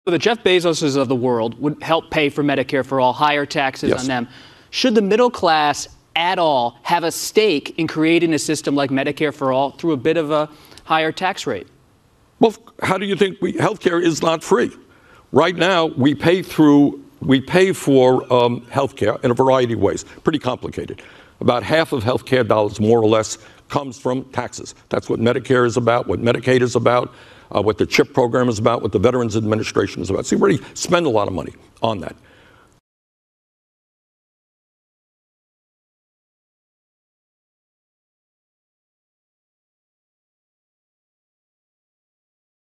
Bernie Sanders Admits Single Payer Means A Middle Class Tax Hike: “Health Care Is Not Free”
Sen. Bernie Sanders admitted to the Washington Post’s Robert Costa that the middle class will experience a tax hike in order to pay for his single-payer plan on 7/16/19.